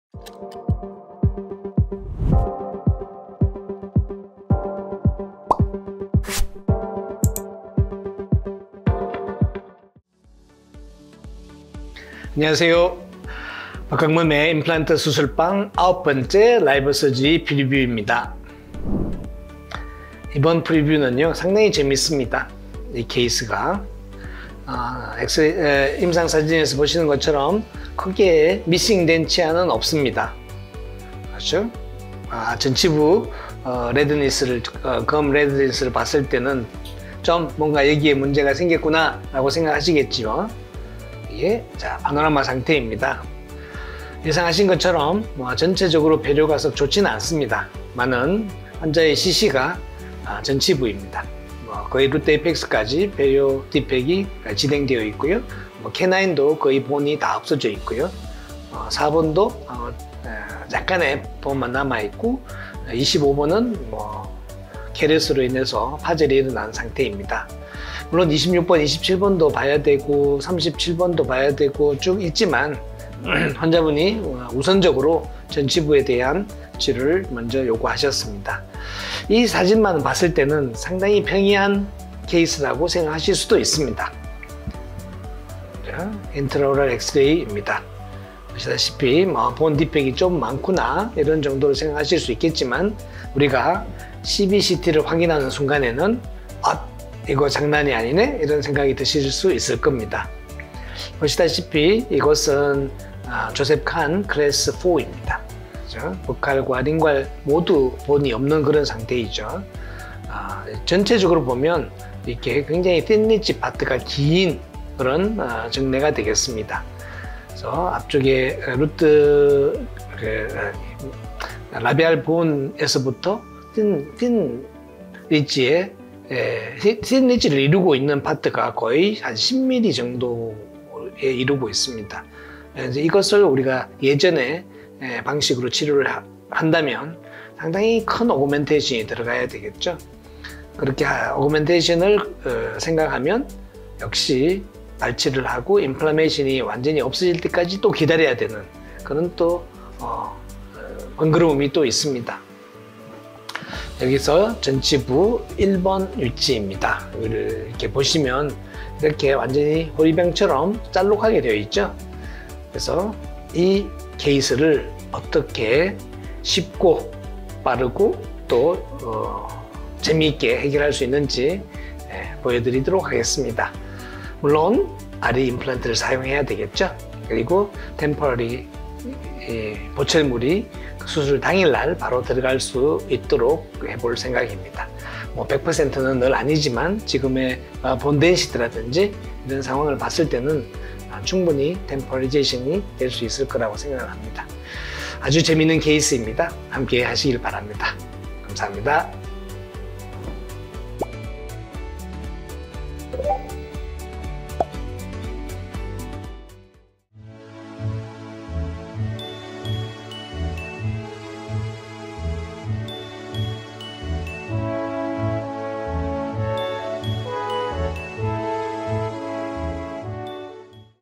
[Preview] Can implants be placed without GBR with thin ridges & bone defect in the upper anterior?
전체적으로 perio가 좋지는 않습니다.
굉장히 thin ridge part가 긴 case가 되겠습니다.
우리가 이것을 예전의 방식으로 치료를 한다면 상당히 큰 augmentation이 들어가야 되겠죠.
그래서 이 case를 어떻게 쉽고 빠르고 또 재미있게 해결할 수 있는지 보여드리도록 하겠습니다.
temporary 보철물은 수술 당일날 바로 들어갈 수 있도록 해볼 생각입니다.

11월 5일 공개되는 9회 최종 보철 편
'Can implants be placed without GBR for patients
with thin ridges and bone defect in the maxillary anterior region?'
많은 관심과 시청 부탁드립니다.